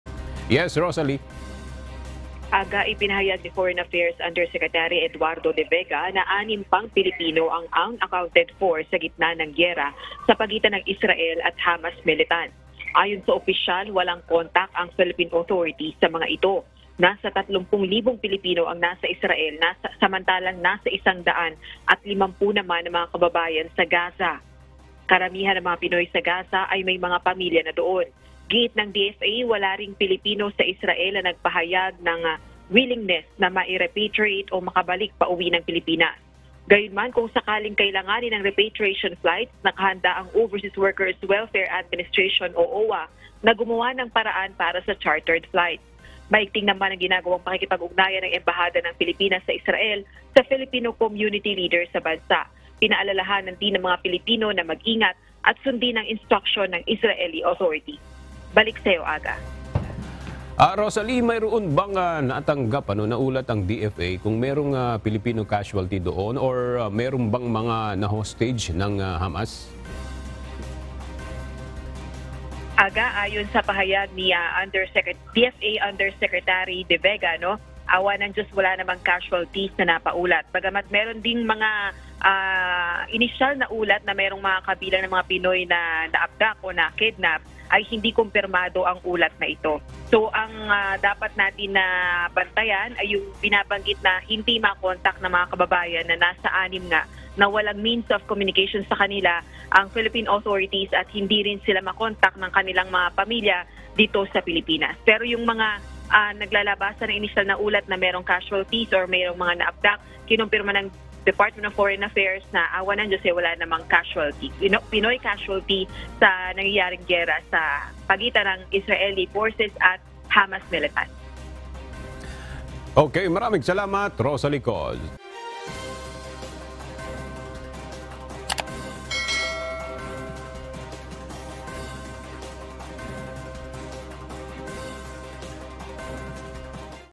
6 Pilipino sa Israel, ‘unaccounted for’ sa gitna ng kaguluhan laban sa Gaza – DFA official
Nasa anim na Pilipino sa Israel ang ‘unaccounted for’ o hindi pa matiyak ang kondisyon matapos mawalan ng contact sa gitna ng giyera, ayon sa Department of Foreign Affairs.